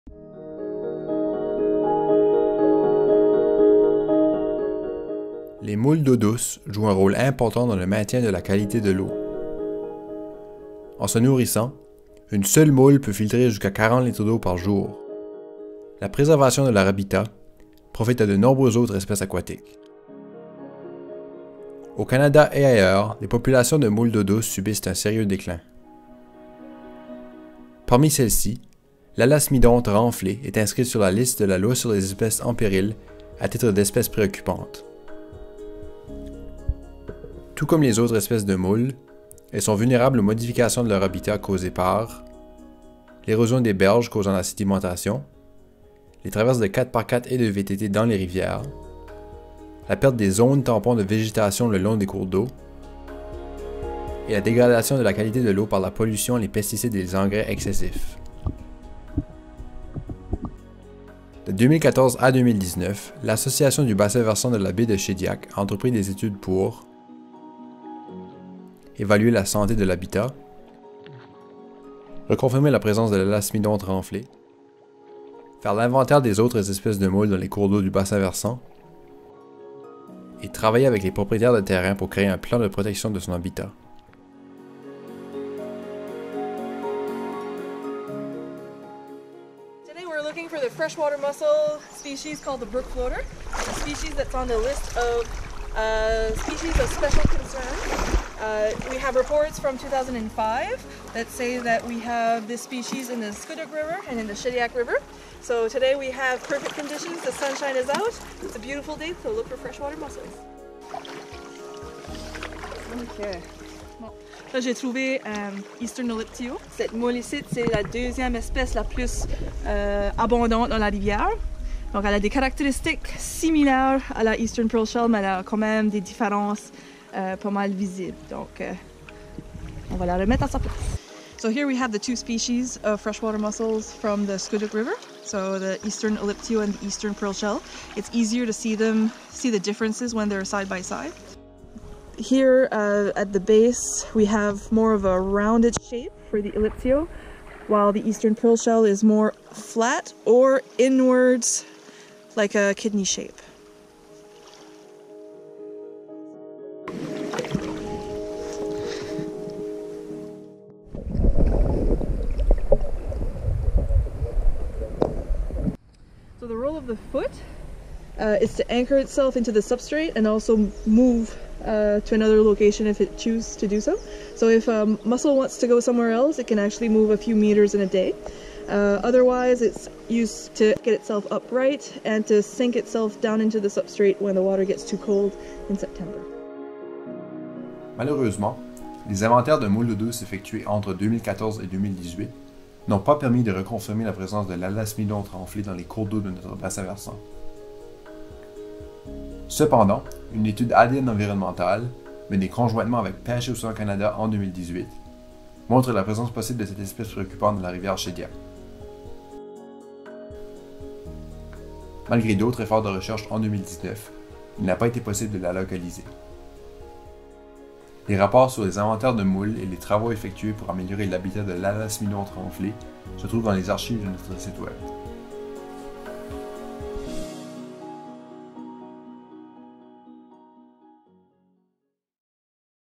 Moules d’eau douce / Freshwater mussels
Les moules d'eau douce sont un élément souvent négligé de la biodiversité d'eau douce et l'un des plus menacés. L’alasmidonte renflée qui se trouve seulement au Nouveau-Brunswick, en Nouvelle-Écosse et au nord-est des États-Unis, est inscrite sur la Liste de la Loi sur les espèces en péril à titre d’espèce préoccupante. Étant donné le déclin de cette espèce, il est important de protéger nos rivières pour assurer la santé de toutes les moules d'eau douce et autres espèces aquatiques.

Freshwater mussels are an often overlooked element of freshwater biodiversity, and one of the most threatened. The Brook Floater, only found in New Brunswick, Nova Scotia and the Northeastern United States, is listed as “Special Concern” under the Species at Risk Act. Given the decline of this species, it is important to protect our rivers to ensure the health of all freshwater mussels and other aquatic species.